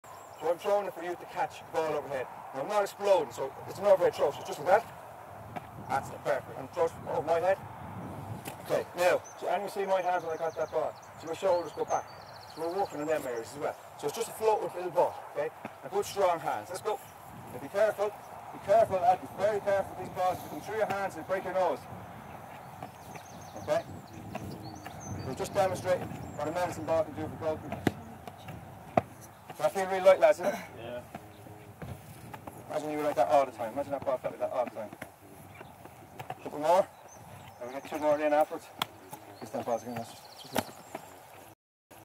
Goalkeeper Warm up Upperbody
Goalkeeper workshop with Colm Nally - Upper-body warm up